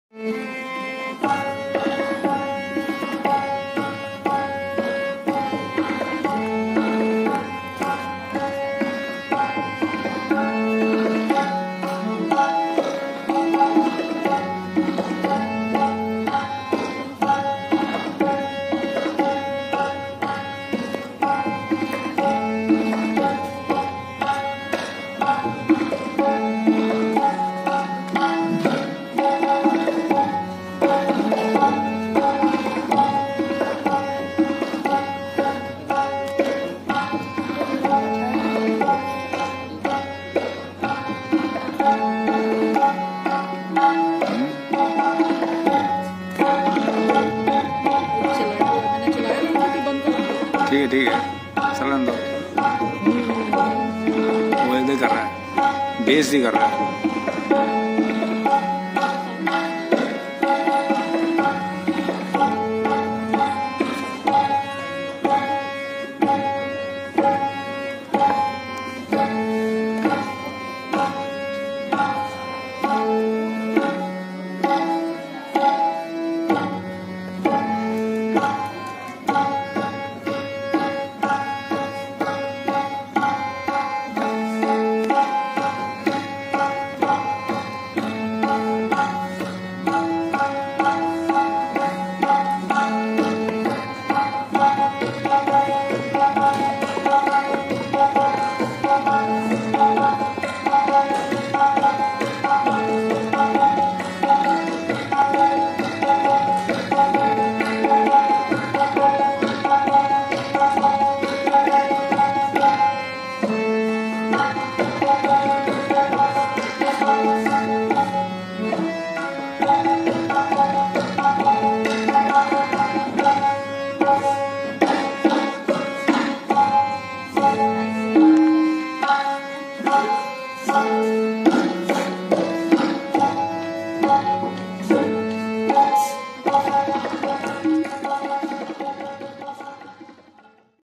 DOE Distric level competition**Solo Pracussion(TABLA)* 🪘 *Krishna* ..🎶🎶🎶🎶got *First Prize*
Congratulations 👏 to all proud members
It's a day of big achievements for GLT SBM , Nehru Nagar👏👏
 Music Department …🎉
*DOE Distric level competition*
*Solo Pracussion(TABLA)*
  🪘 *Krishna*  ..🎶🎶🎶🎶got *First Prize*🥇 🎶🎶🎶🎶
He will represent school at state level.

Congratulations to  GLT Family,students,mentors and parents.👏👏